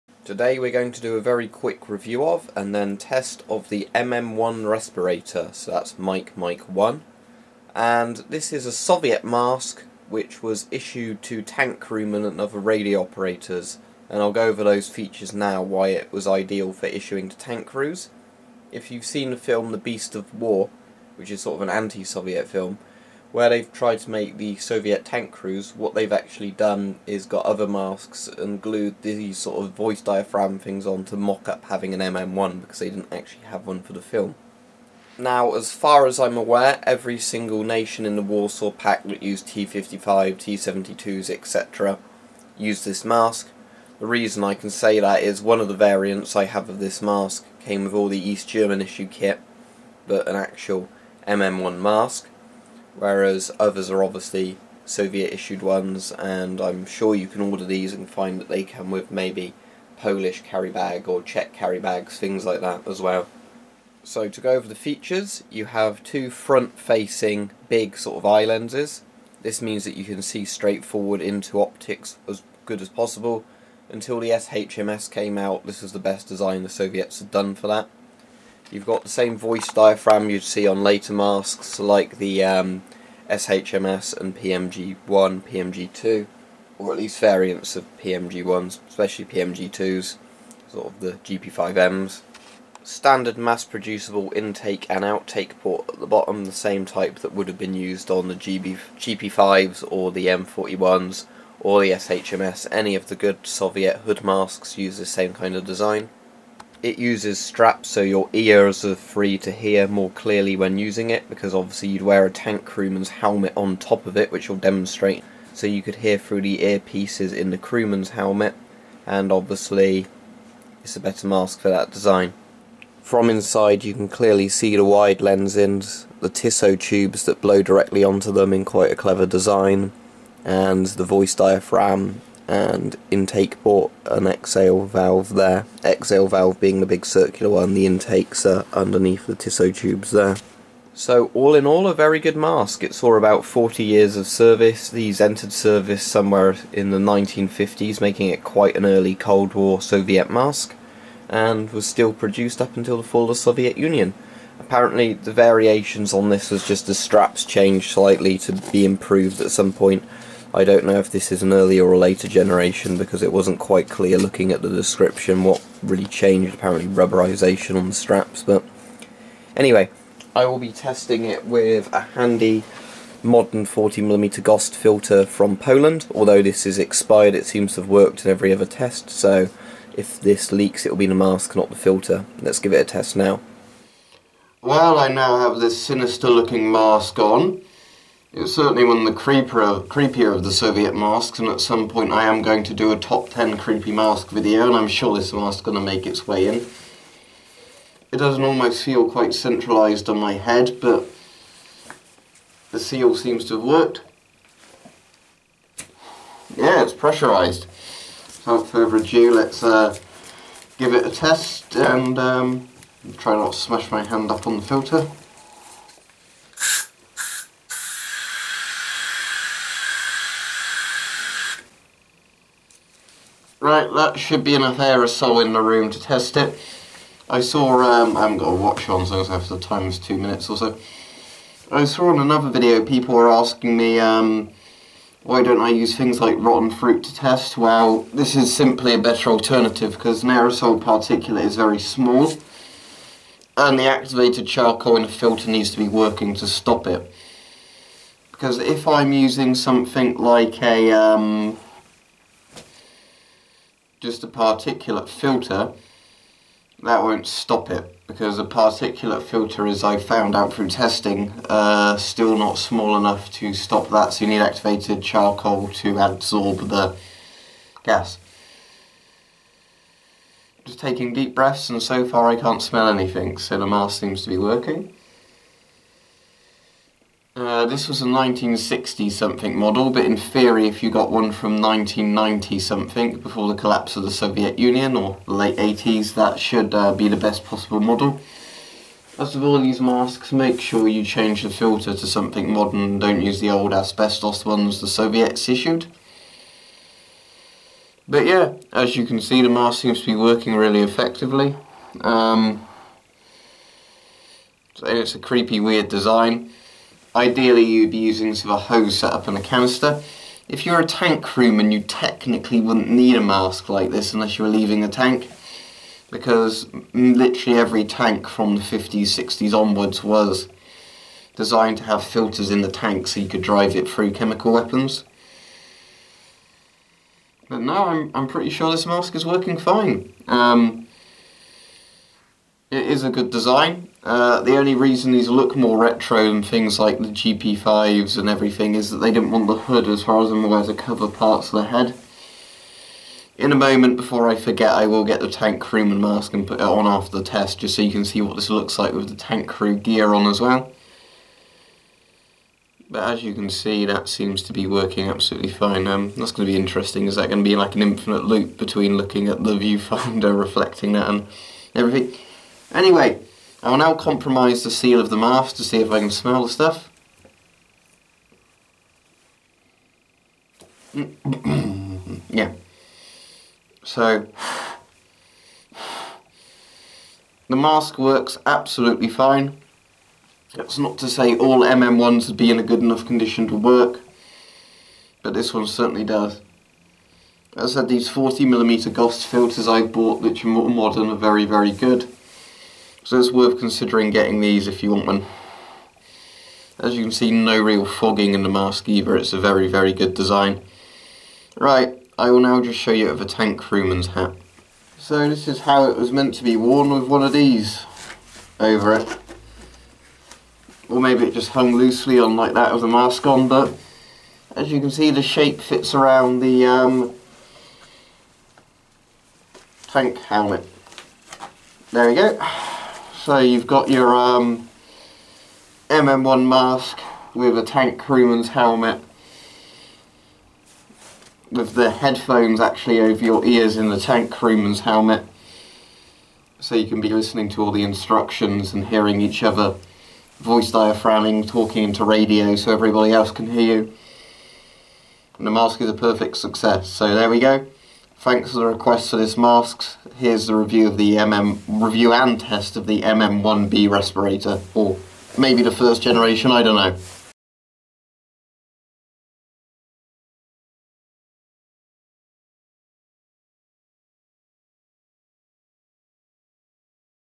MM1 Gas Mask Review and Test
Review and Test of the MM1 Gas Mask/Respirator.
The Soviet MM1 was used by both the Red Army and other Warsaw Pact allies. The mask uses a rubber facepiece with two front facing eye lenses, a voice diaphragm and an effective Tissot tube system to prevent the mask from fogging up.
The idea behind the mask was to be used by tank crewmen and radio operators who needed front facing eyepieces to look through optics and speak clearly into radios. The mask seems to be a design influence on the ShMS/SChMS gas mask, however supposedly the MM1 was used until the collapse of the Soviet Union, mostly because it was easier to hear wearing one than the helmet style rubber hood masks.